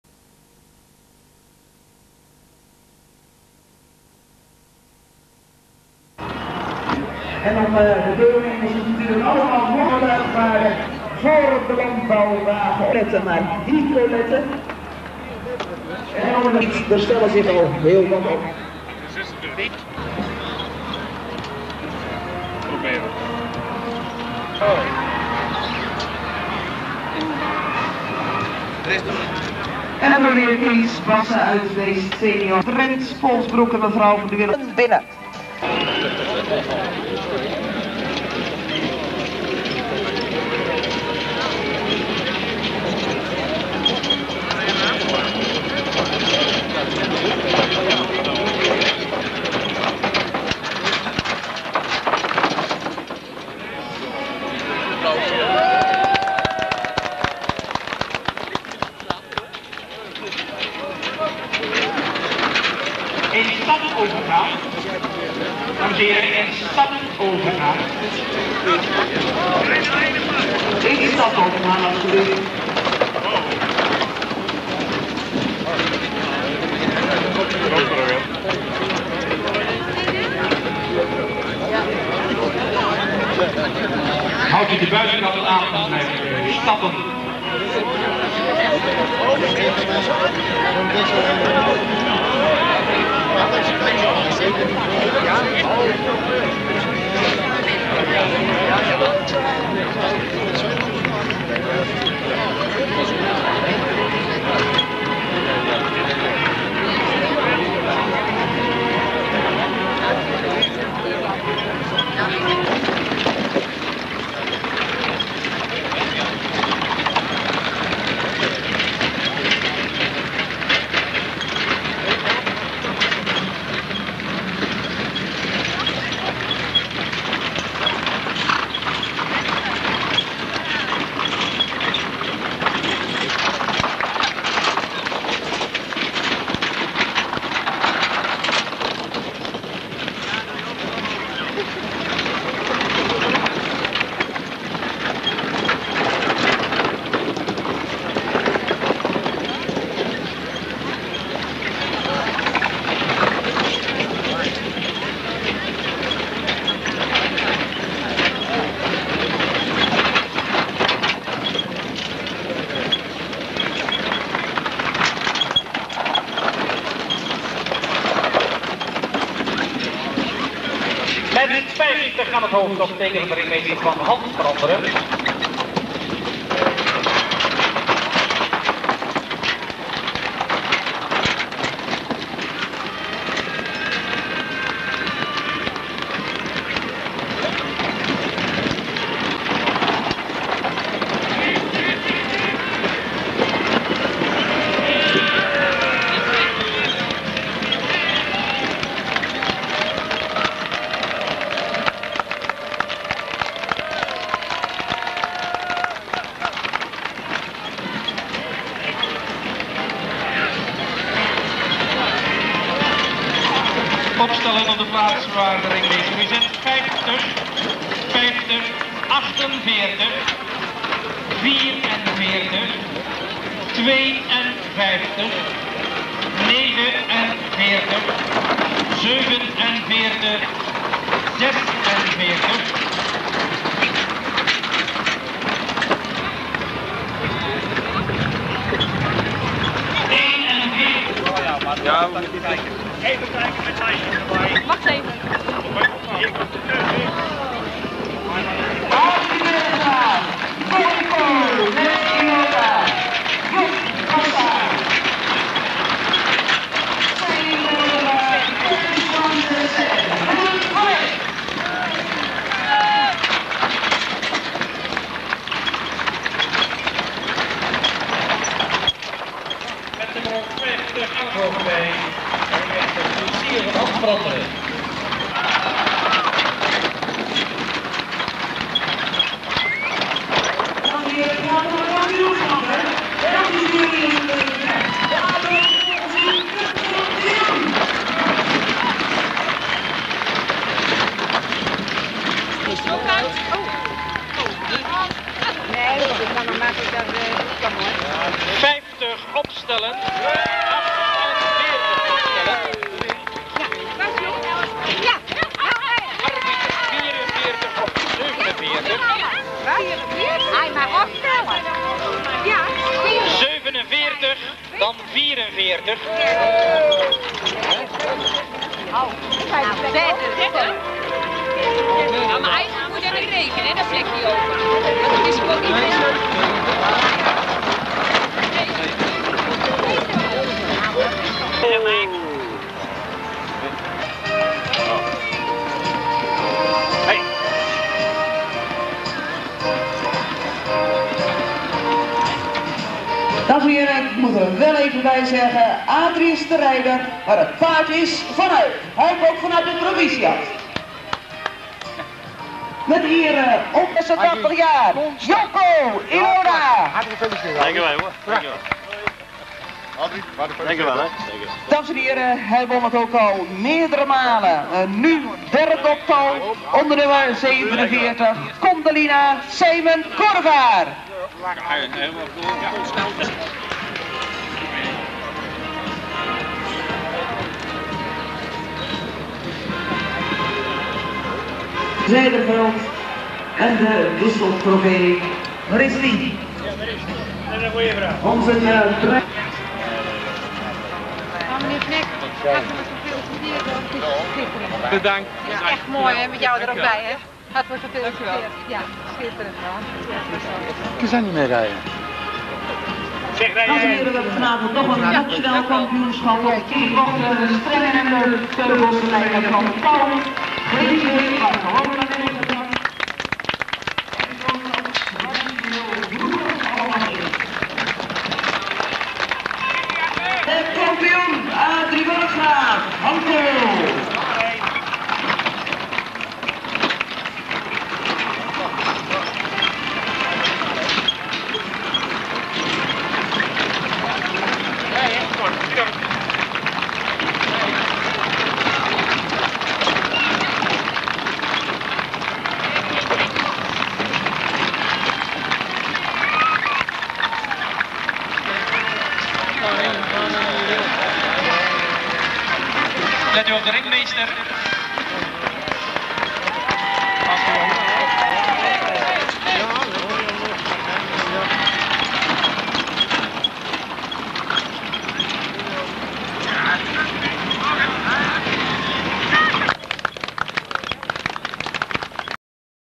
Kampioenschap luchtbandenwagen, CH Hagestein 1999
CH Hagestein 1999

Kampioenschap éénspannen voor de luchtbandenwagen
1. Hanco (v. Wilhelmus) van Huib Burggraaff met Adrie Burggraaf
2. Ilonia (v. Dobas) met Joop Kool
3. Condalina (v. Renovo) met Sijmen Korevaar
4. Job Bassa
5. Lambala (v. Fabricius) met Teus van Zessen
Schoonste geheel: Willem van Eck

Overige deelnemers:
Frits van Muyden met Bicolette (v. Triomfator)
Cobi Bassa-Van Muyden met Hicolette (v. Bravoure)

(From the archives of 'Het Tuigpaard / The Dutch Harness Horse Collection', takeover not allowed)